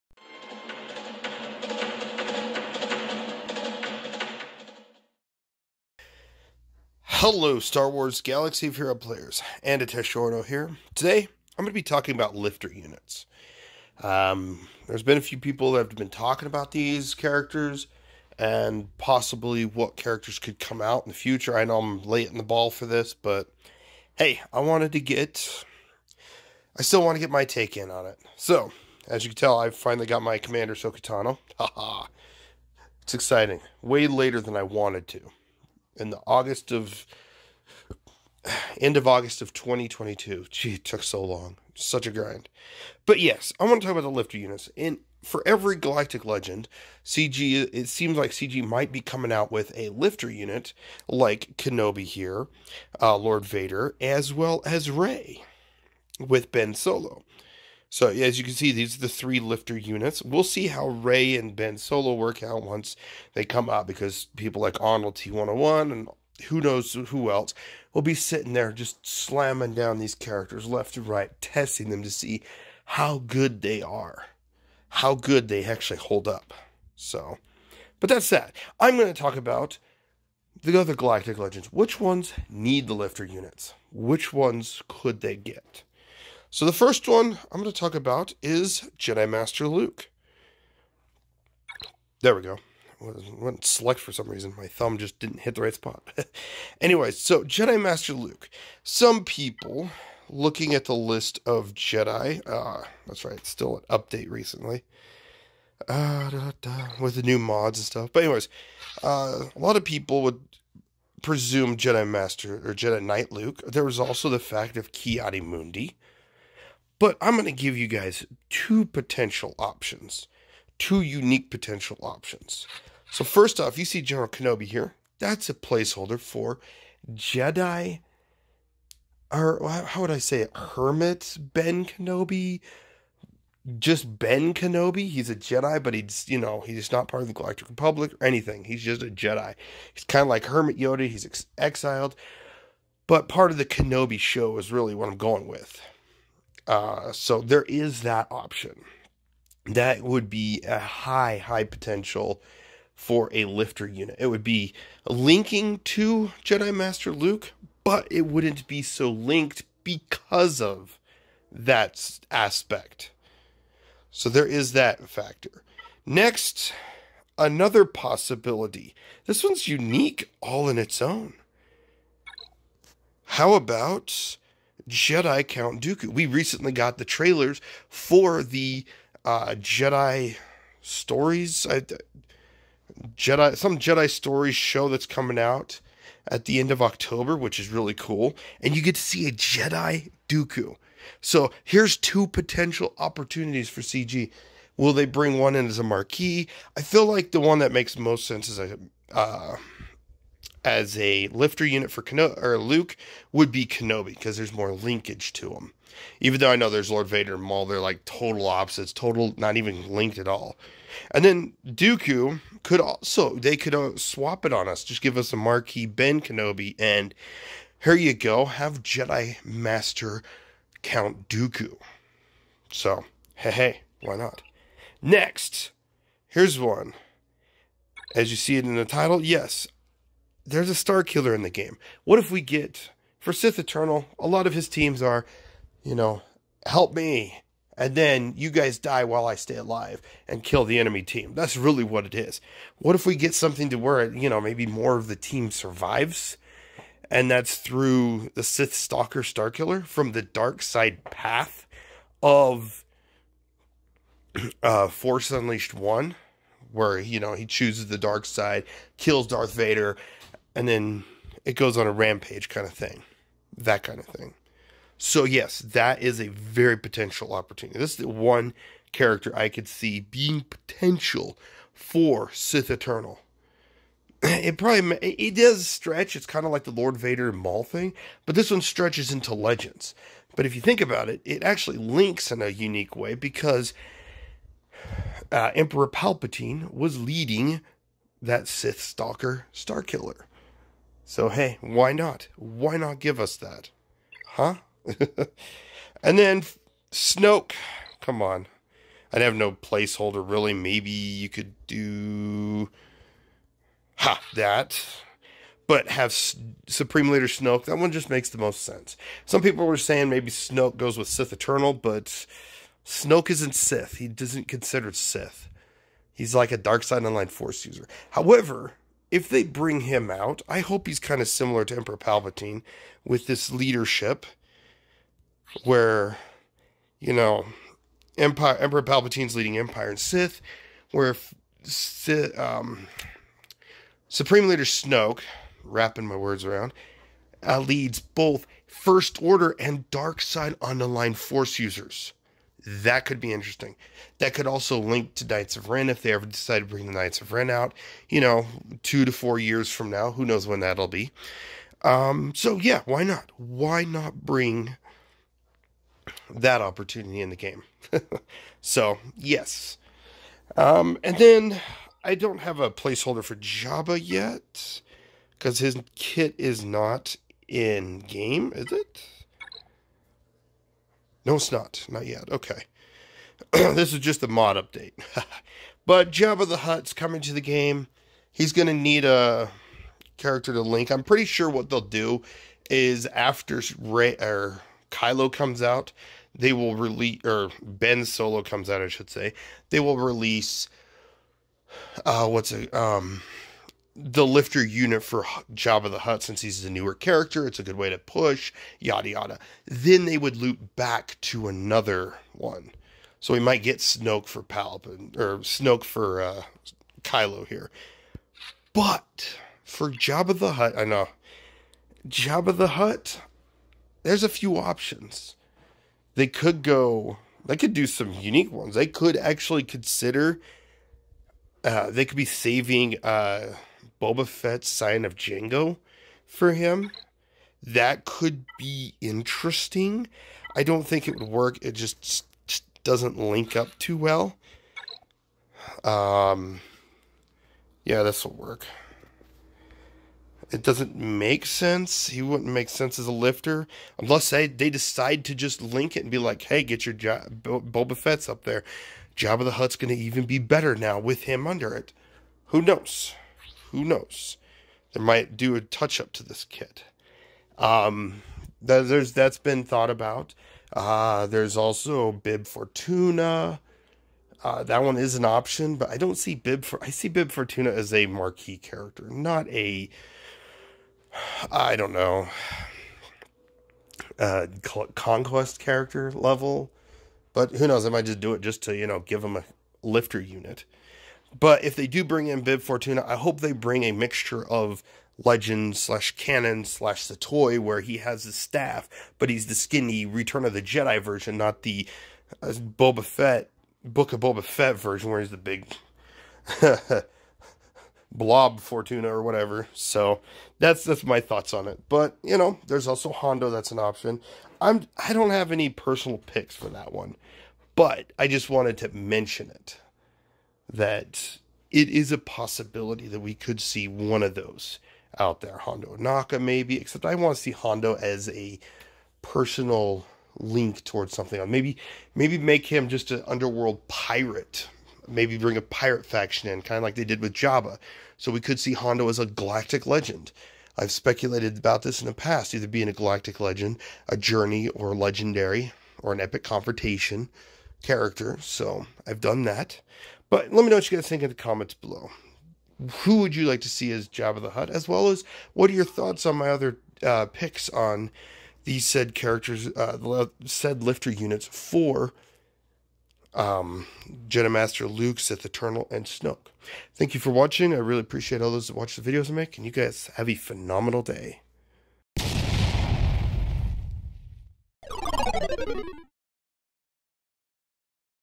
SWGOH: Galactic Legend Lifter Unit Predictions!!!
Here are my thoughts on lifter units. Hope we get to see some of these as the actual lifter units for these factions!!!